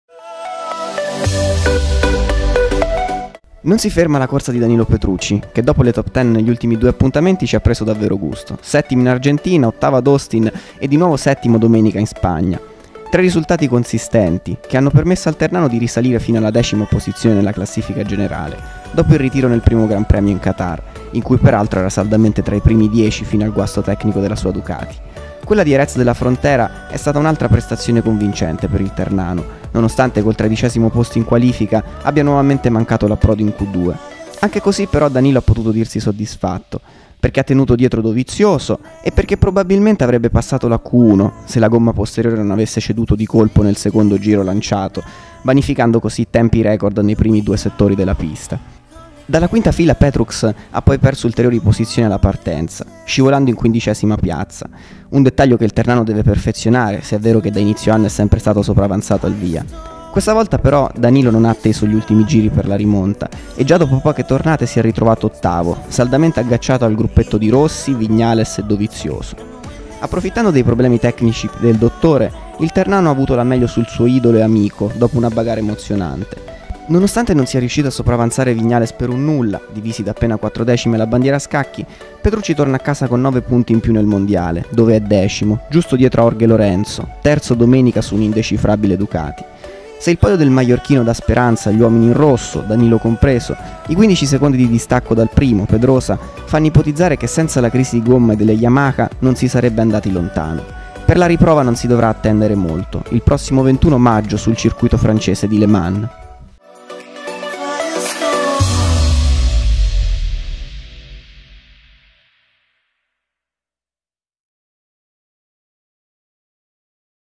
MOTOGP: TOP TEN DI PETRUCCI A JEREZ
Il servizio di Giulio Sacco, che sintetizza il Gp spagnolo di Jerez de la Frontera, in cui Danilo Petrucci ha conquistato per la terza volta consecutiva un piazzamento nei primi 10.